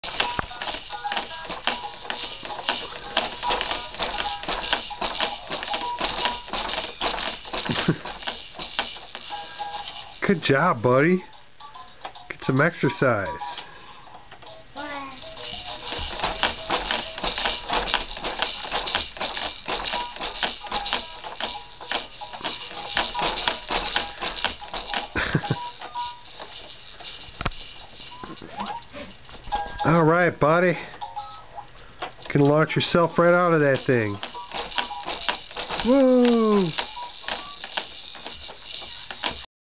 Carter loves his bouncy.
I wonder how much his brain jostles around inside his little skull when he does this.